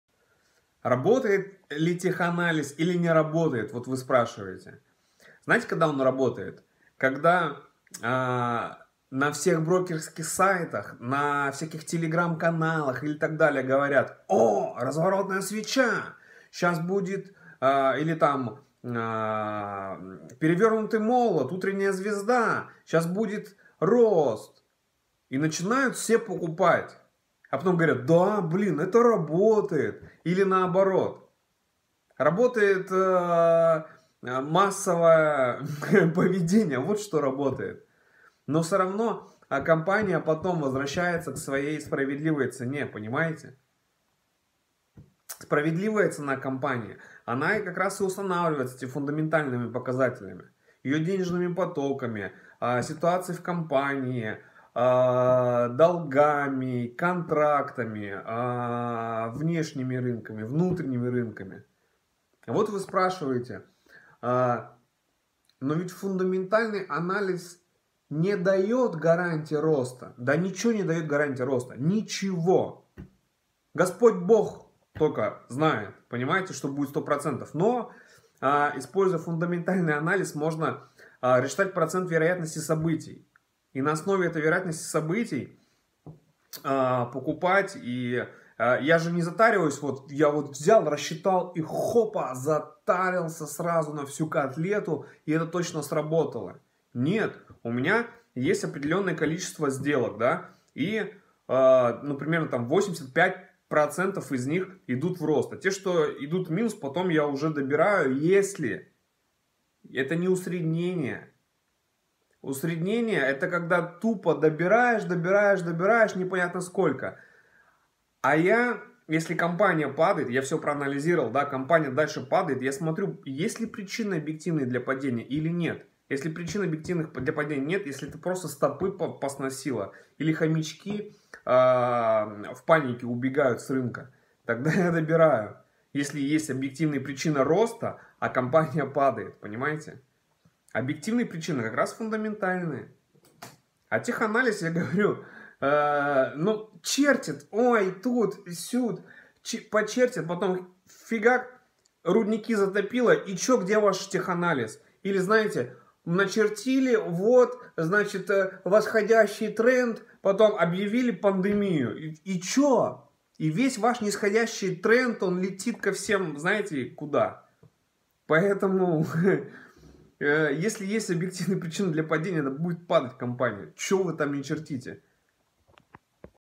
Теханализ или фундаментал ? Что использую я при инвестировании.
Тех анализ ))) сколько хомячьих шкурок было ободрано в надежде на разворотрый тренд) как делаю я - в этом видео .